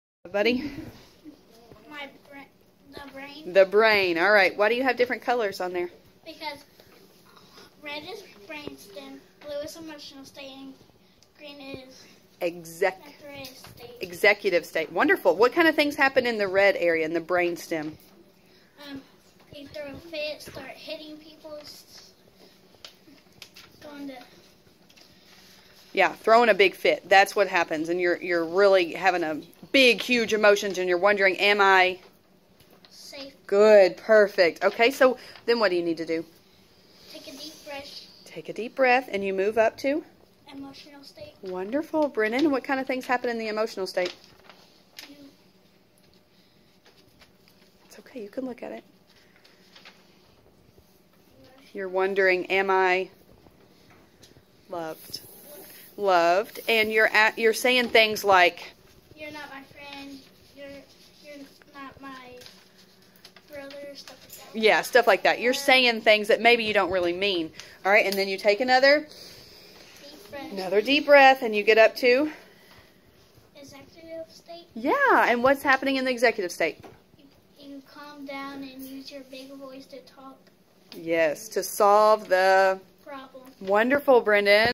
2nd grader explains Brain State model
At Estes elementary we are learning what happens in your brain when you experience any upset.